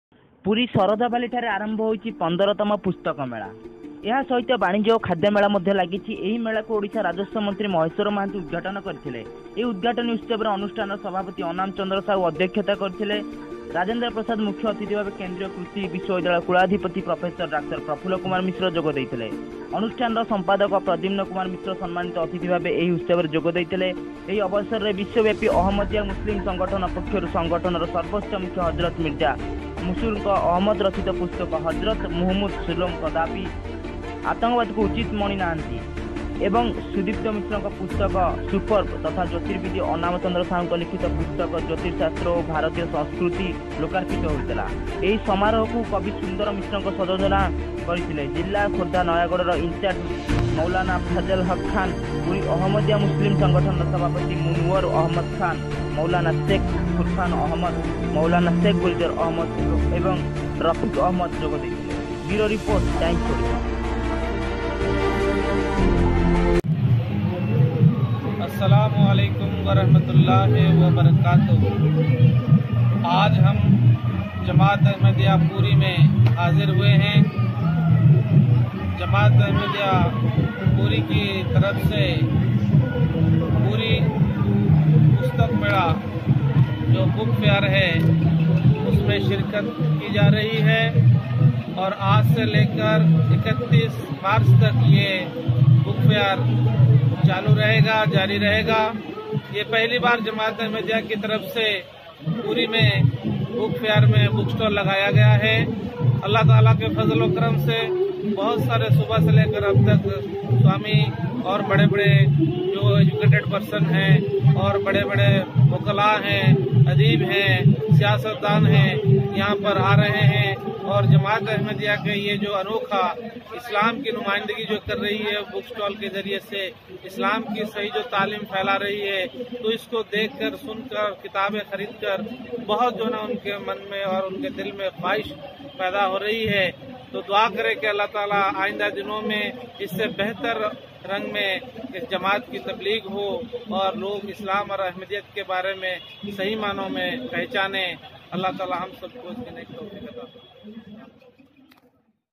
Ahmadiyya Muslim Jama'at Puri participated in Puri Book Fair
Ahmadiyya Muslim Jama'at Puri participated in Puri Book Fair to spread the true teachings of Islam covered by Times Odia

video courtesy Times Odiya